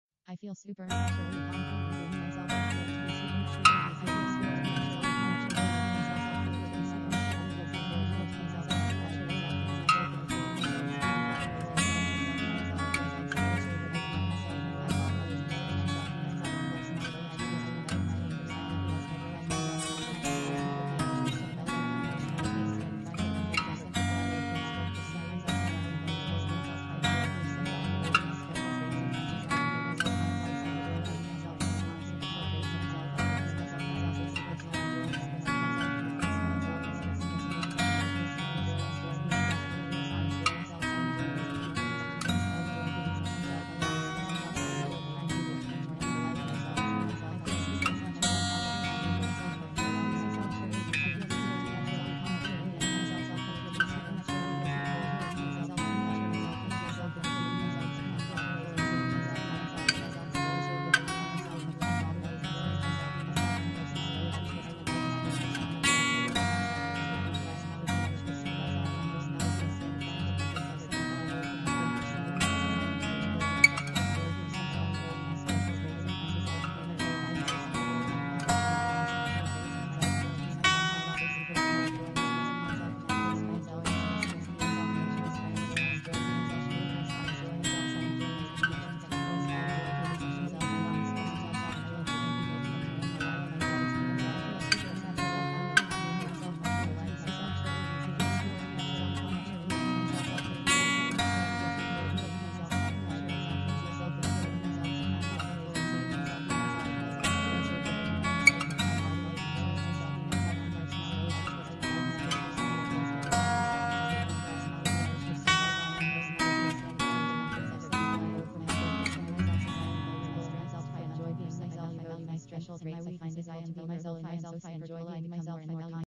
Confidence about being yourself | subliminal
Confidence about being yourself:
-  it's supernaturally easy for you to be yourself
-  grow your personality instead of trying to be like others
-  feel good in your own skin
-  know your strength and use them to the fullest
-  feel ok about your weaknesses, stay vulnerable
-  enjoy expressing your feelings and emotions
-  enjoy introducing yourself, making people to get to know you
-  find ways to overcome difficulties as yourself